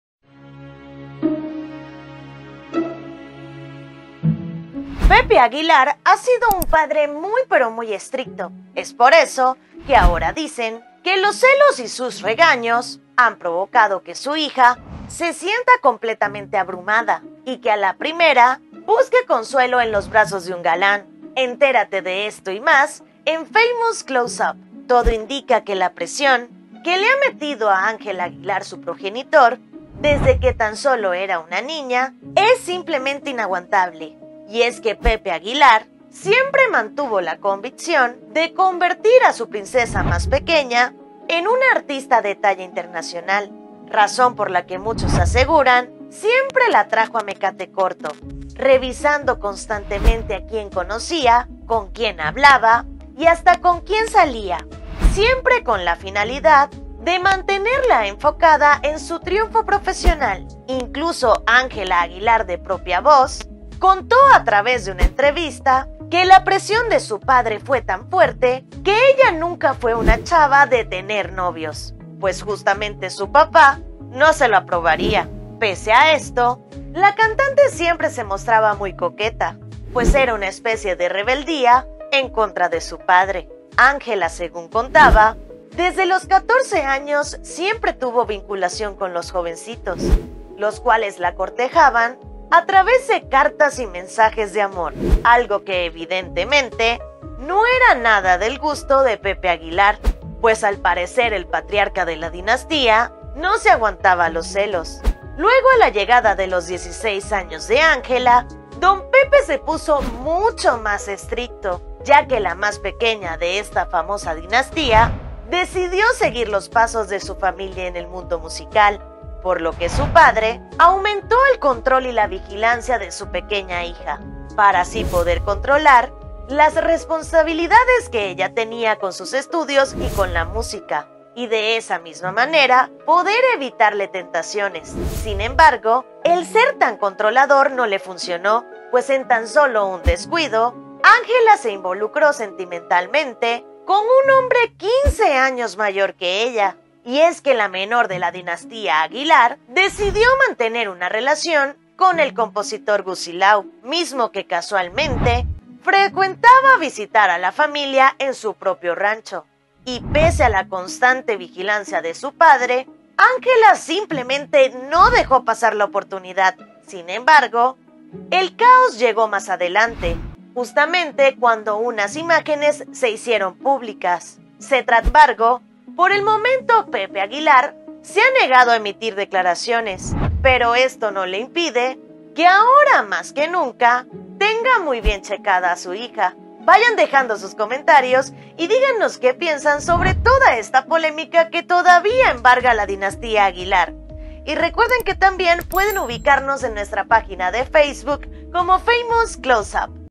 🔴😱 Papá de Ángela Aguilar la orilló a ser coqueta
Pepe Aguilar era muy sobreprotector y ella busca la salida siendo coqueta…